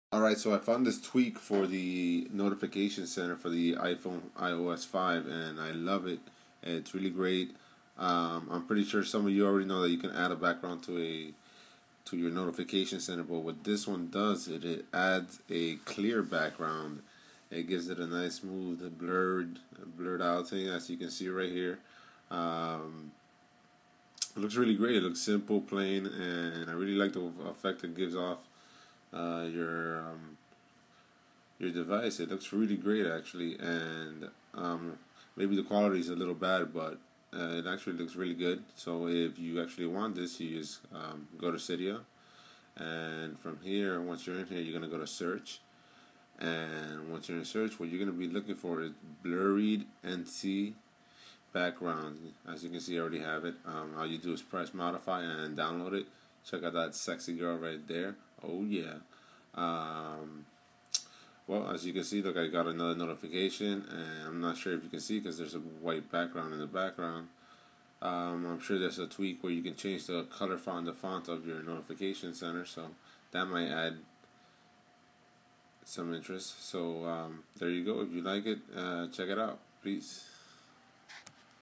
New iPhone 4S iOS 5-5.0.1 Tweak Clear Notification Background Wallpaper
Top Beautiful Blurry Notification Center Theme for iPhone4 and iphone4s or just ios5. Simple & Elegant. Tweaks in cydia